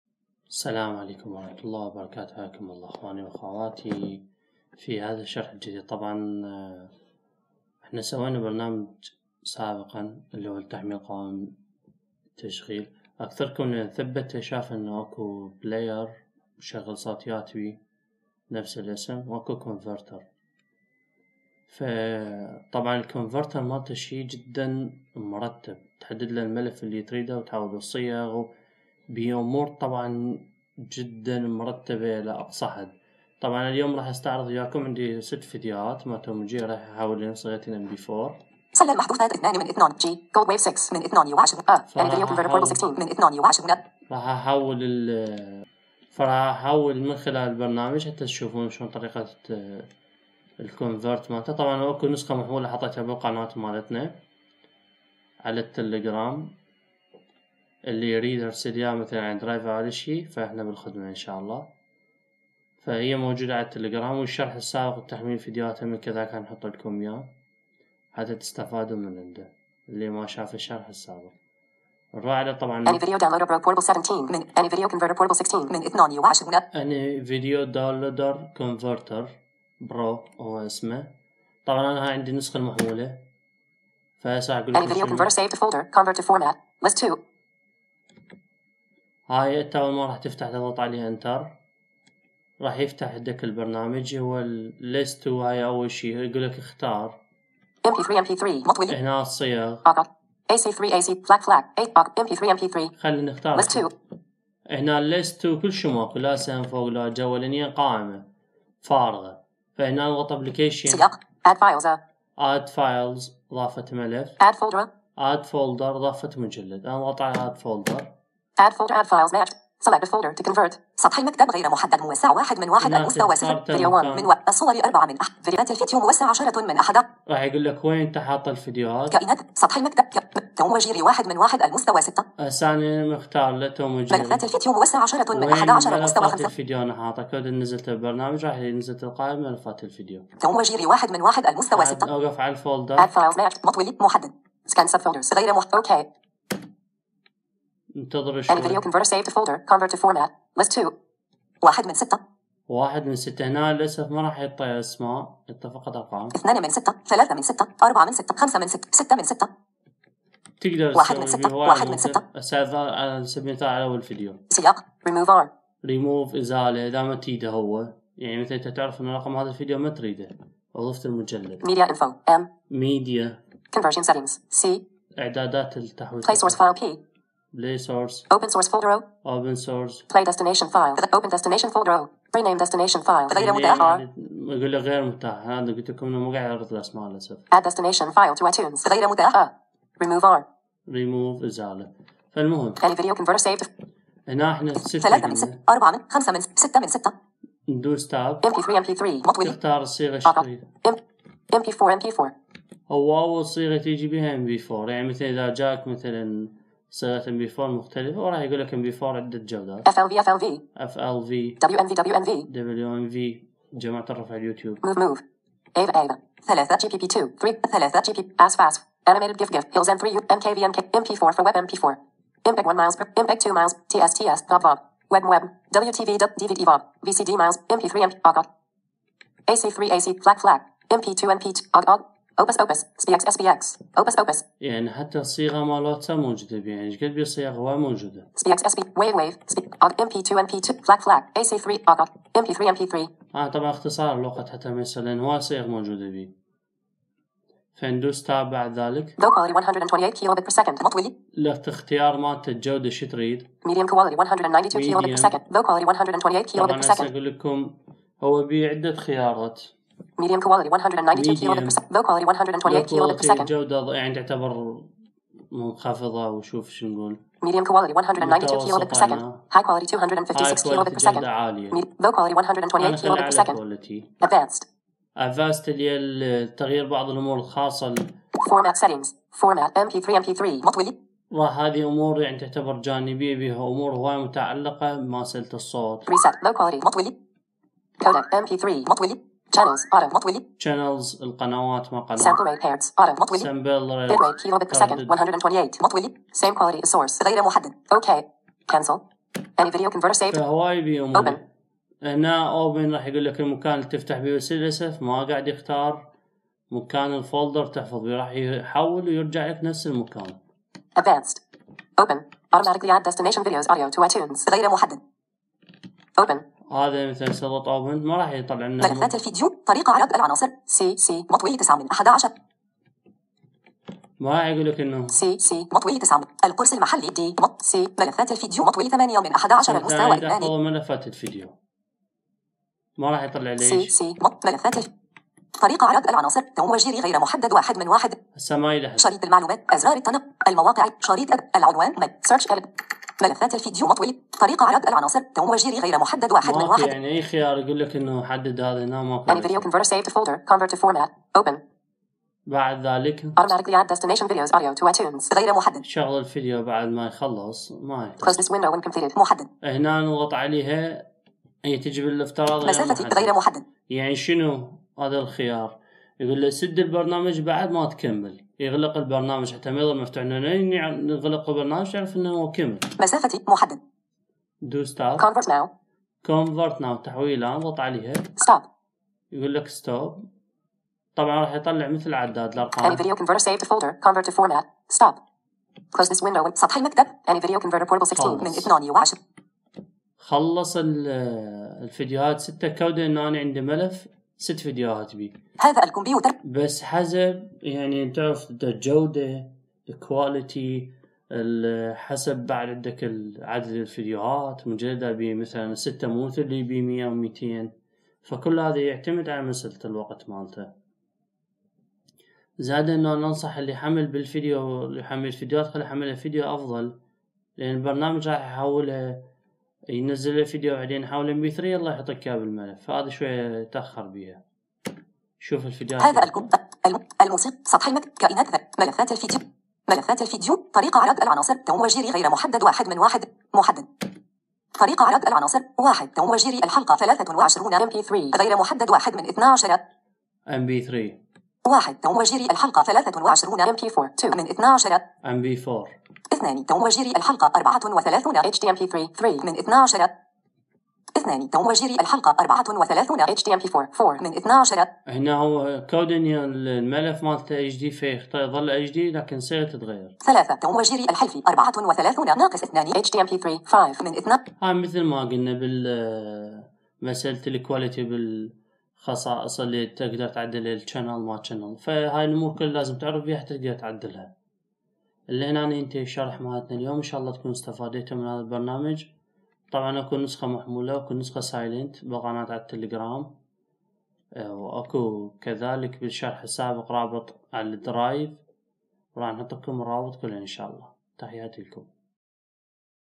شرح برنامج any video converter لتحويل صيغ الفيديو الاستاذ عمار صالح￼
رابط الشرح السابق

تحميل البرنامج عن طريق قناة التليجرام

النسخة المحمولة من التلكرام